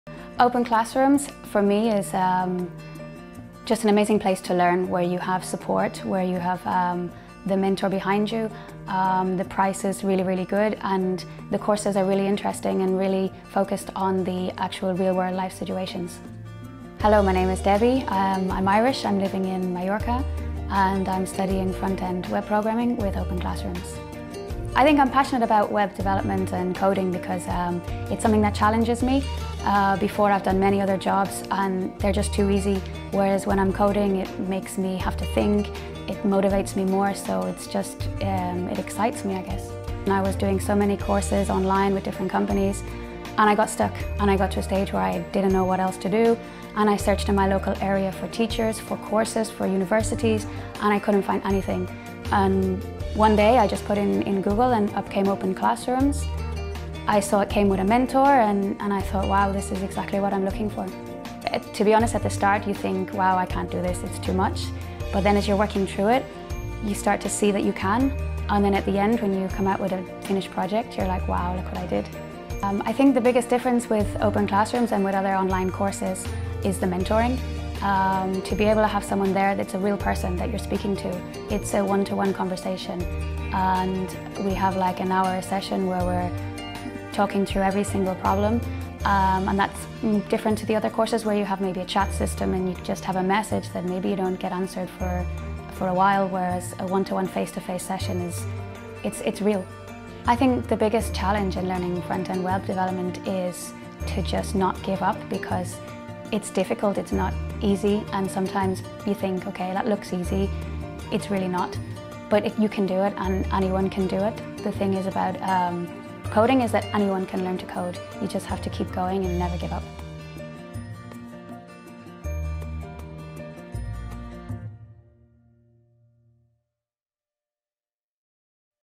CODING: How To Overcome The Biggest Obstacle When Learning Web Development
Debbie is studying a diploma in Front-End Web Development and talks to us about her experience learning to code online. 📢 Our Junior Web Developer path is now open for pre-registration!

Thanks to regular one-to-one online meetings with her mentor, Debbie was able to overcome one of her biggest challenges with learning online: the lack of human support.

OpenClassrooms guarantees you'll find a job within the 6 months following your graduation, or your money back.